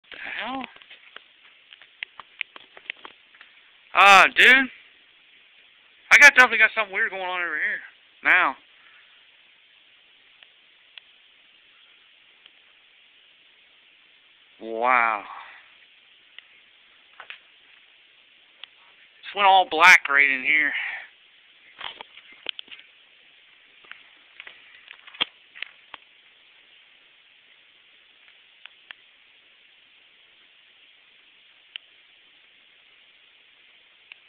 paranormal at lime stone kelms canoo creek.3g2
wild energies and spirit encounters may 22nd 2012 cannoe creek pa wasnt planning on doing paranormal run but experencied quite a trip